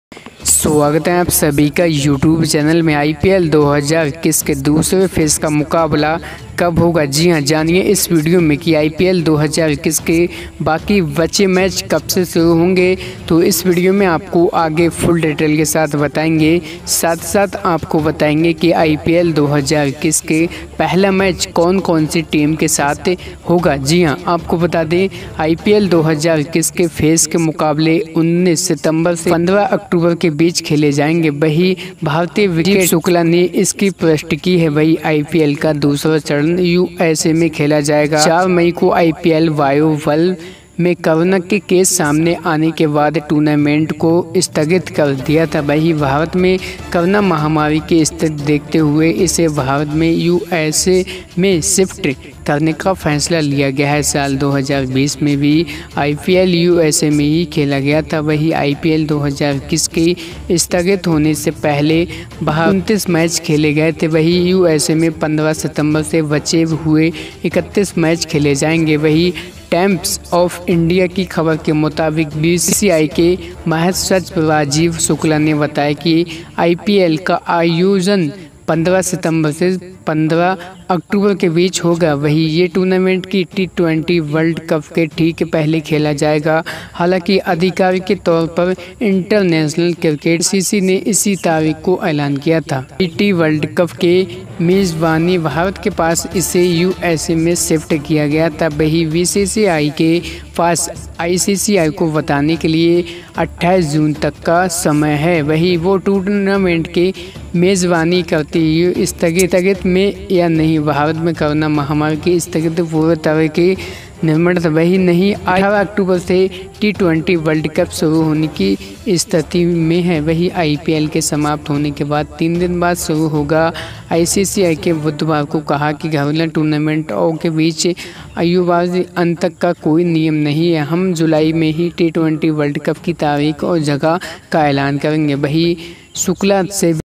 Chennai Super king Squad IPL 2021 l देखिए IPL के बदली पूरी टीम , धोनी , ने करे 3 बड़े बदलाव
Chennai Super Kings Squad IPL 2021 | देखिए IPL के लिए बदली चेन्नई की पूरी टीम,धोनी न करे 3 बड़े बदलाव